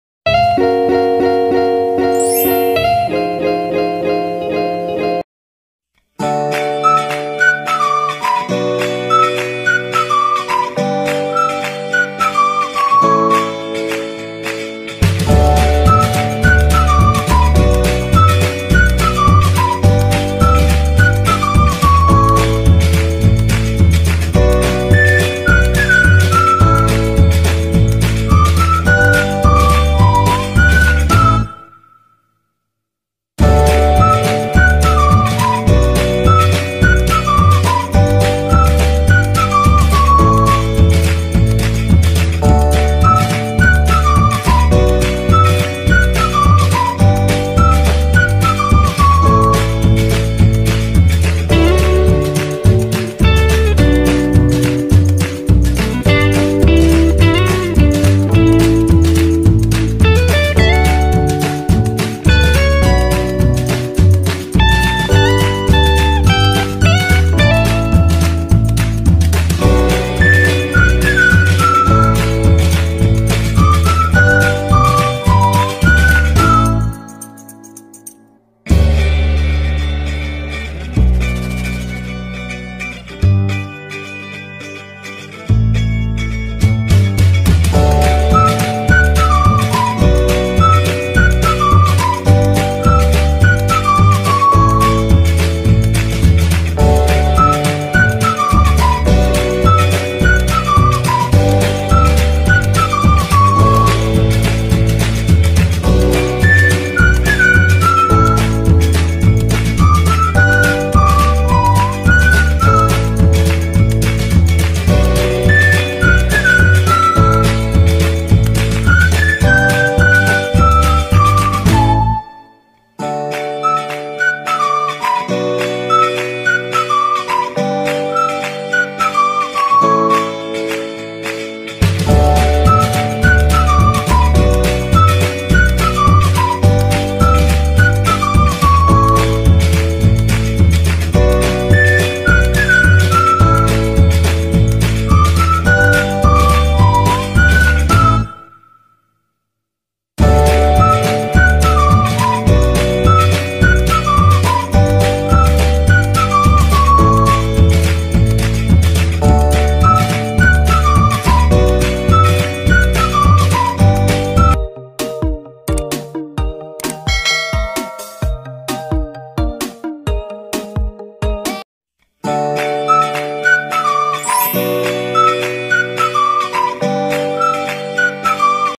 POTATO CHEESE ROLL|EDNA'S KITCHEN|EPISODE:25
INGREDIENTS:
CHEESE 165 GRAMS
PATATAS 2 PCS.
OIL
YOUR CHOICE OF DIP